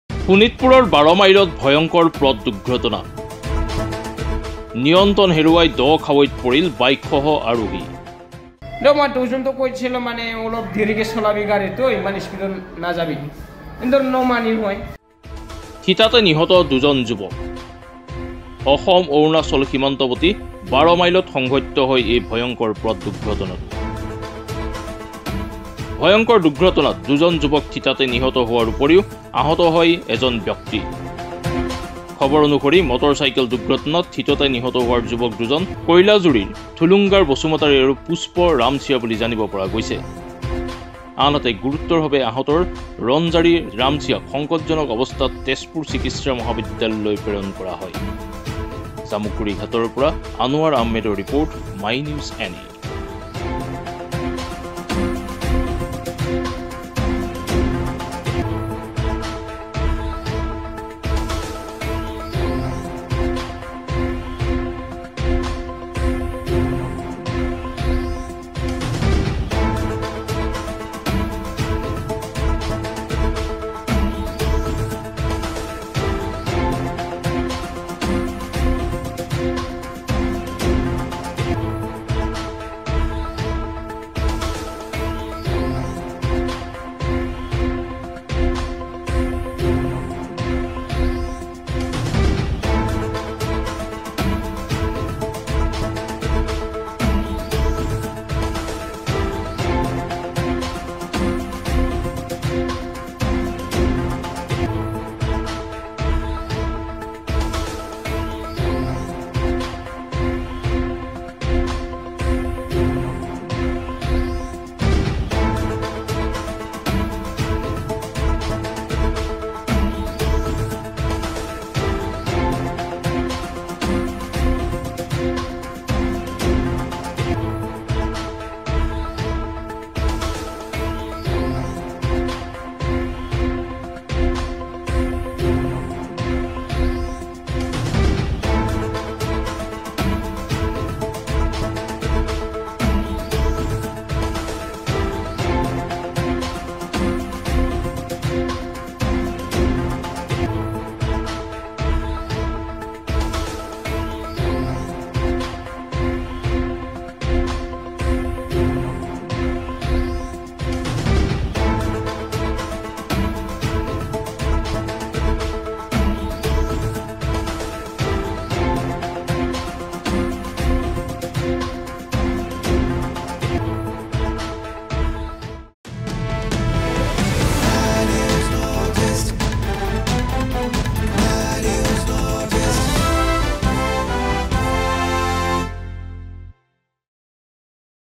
🔴শোণিতপুৰৰ বাৰ মাইলত ভয়ংকৰ পথ দুৰ্ঘটনা
My News NE:  🔴শোণিতপুৰৰ বাৰ মাইলত ভয়ংকৰ পথ দুৰ্ঘটনা
🔴নিয়ন্ত্ৰন হেৰুৱাই দ-খাৱৈত পৰিল বাইকসহ আৰোহী